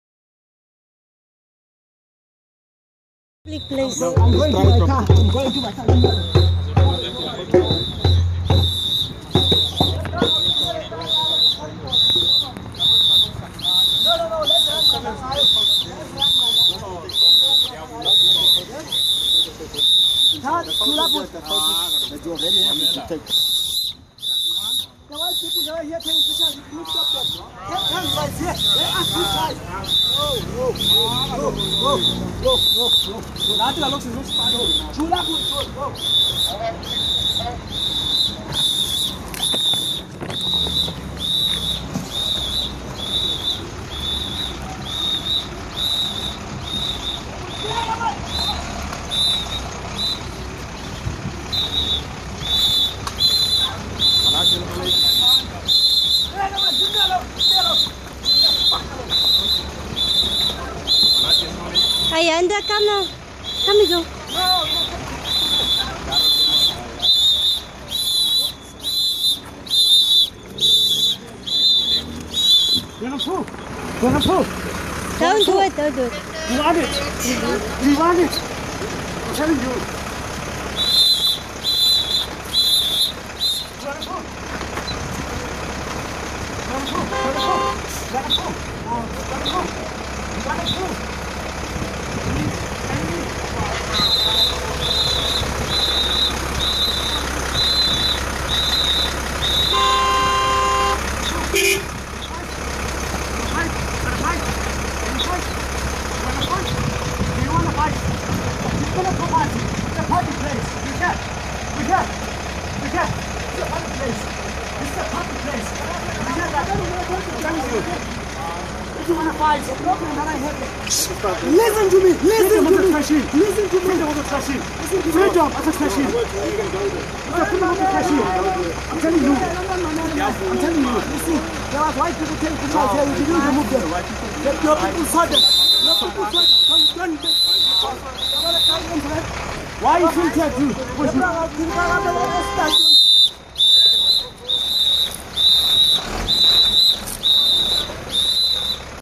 UCT Campus Protection Services Manhandle Photographer
UCT student Chumani Maxwele protested by throwing human excrement at the Cecil John Rhodes statue at the campus. UCT's Campus Protection Services prevented Daily Voice photographer Ayanda Ndamane from covering the protest while dozens of students took videos and photos with their cellphones.